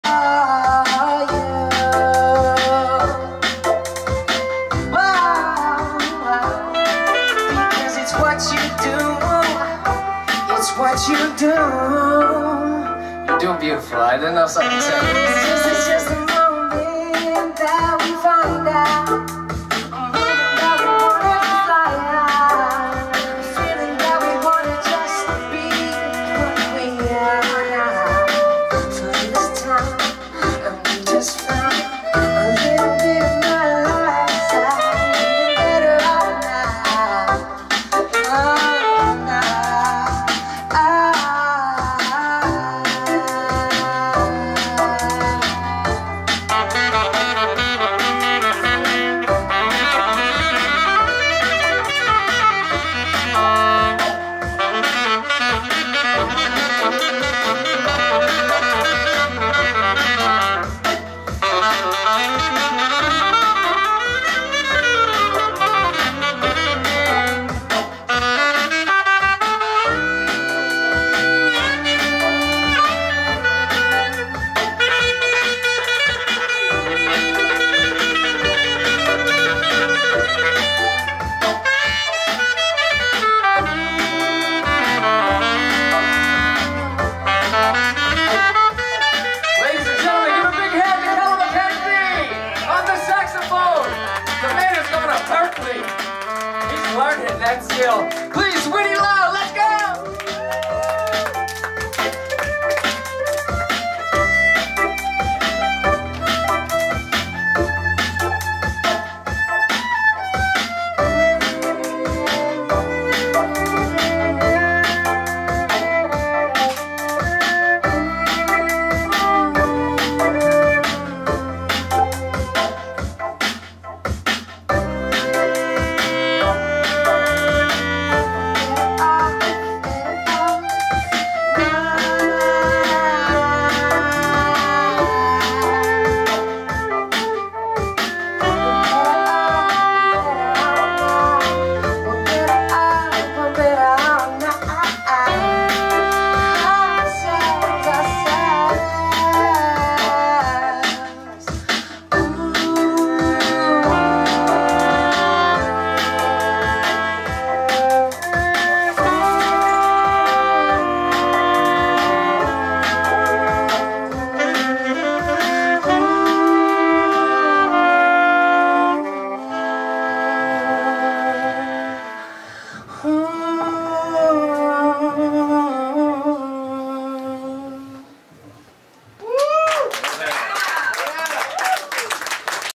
The POP Experiment // Sax + Violin JAM // LIVE NYC
LIVE The Anello Collaboration w Callum McKenzie and Winnie Lau